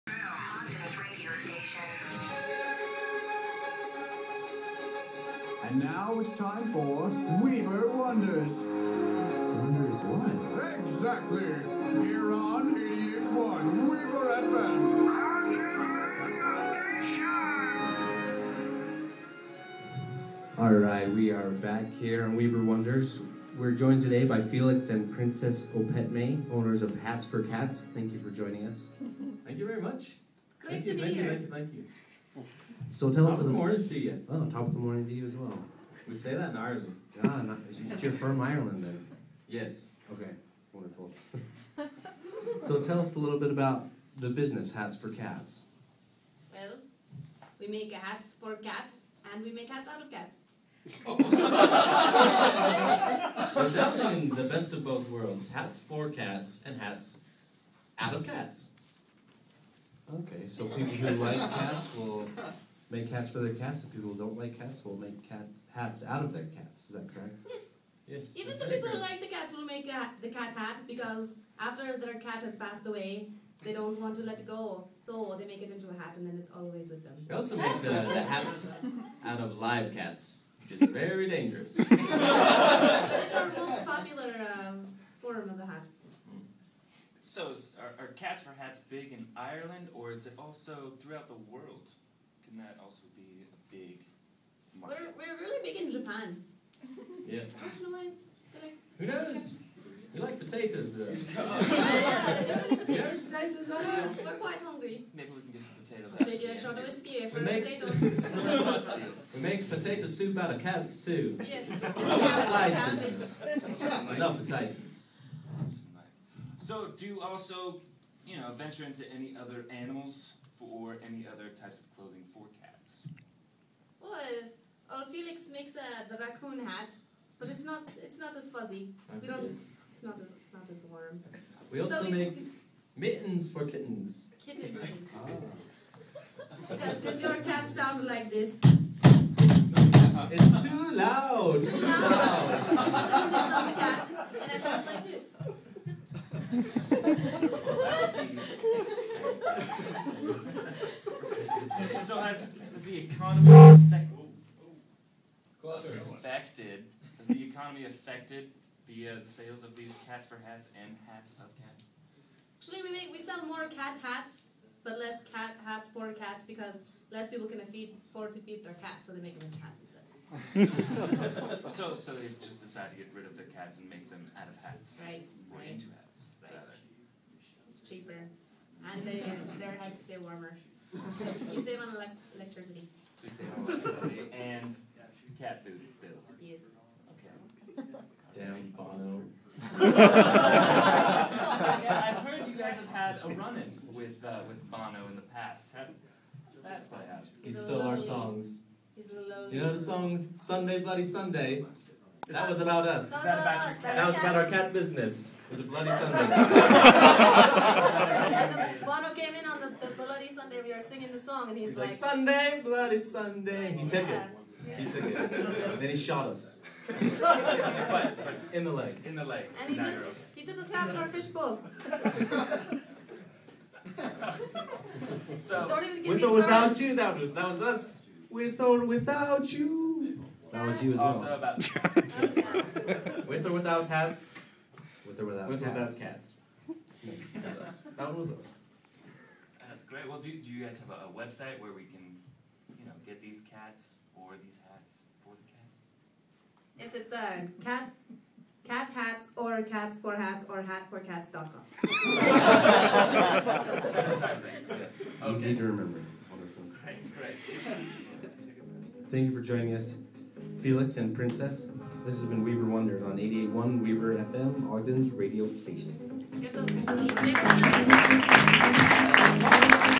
KWCR Interview Class - Hats For Cats
The DJ class, COMM 2730, learns how to properly conduct a professional interview.